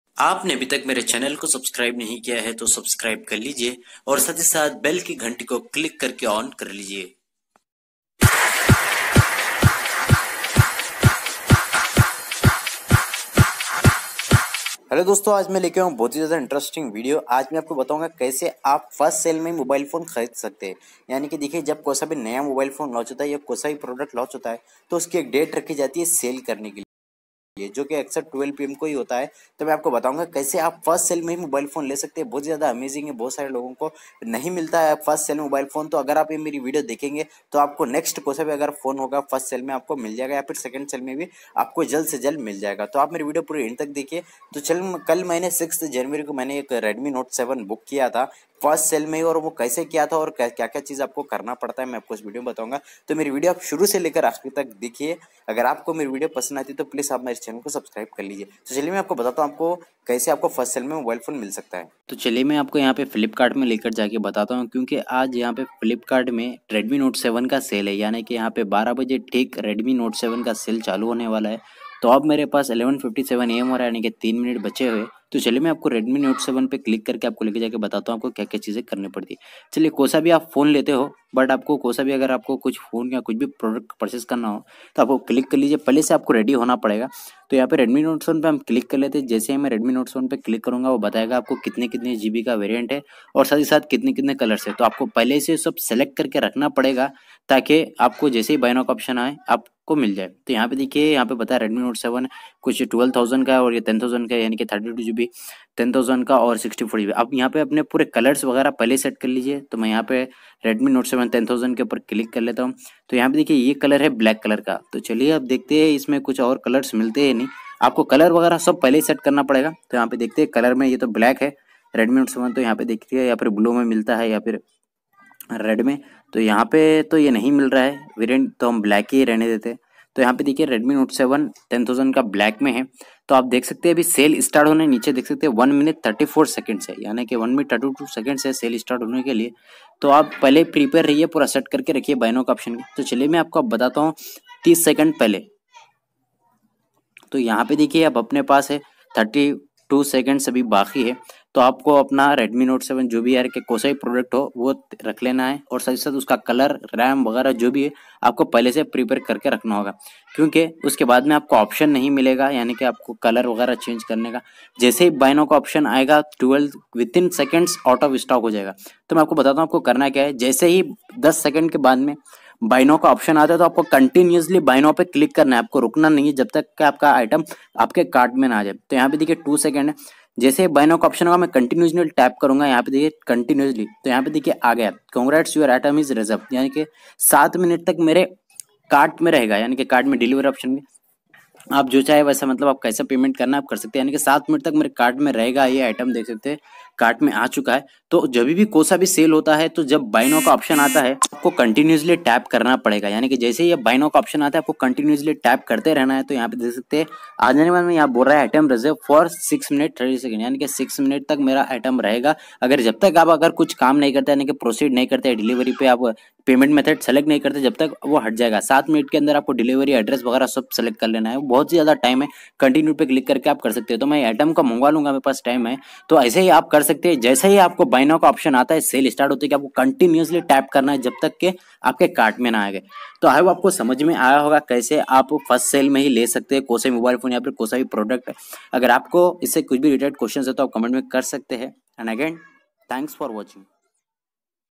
How To Buy Mobile Phones Fast On First Sale In Flipkart
Hello Friends Now I am Back with An Amazing Video Now Today
I am Showing You How To Buy Mobile Phones Fast On First Sale In
Flipkart Its Very Easy You Just Follow My Steps First You Need To Select Your Brand And Colour Before Sale Starts And Prepare Everything Before Sale Start Ok For Better Understanding Watch My Video Till End If You Like My Video Then Please Subscribe